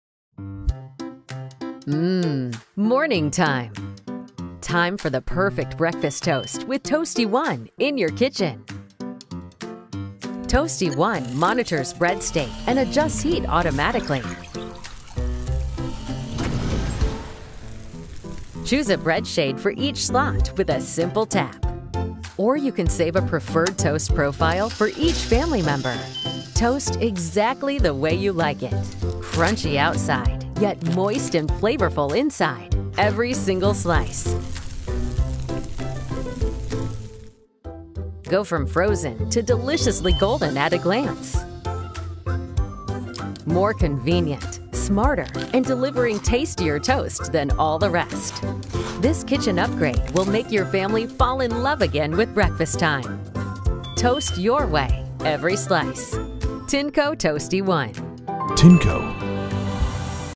Introducing Tineco Toasty One - Toast your way, every slice
Hey foodies, your perfect toast is finally here!
√ Smart heating
√ Slot customization
√ Save your faves
√ Touch screen
√ Shading slider
Check more “yummy” features on @tinecokitchen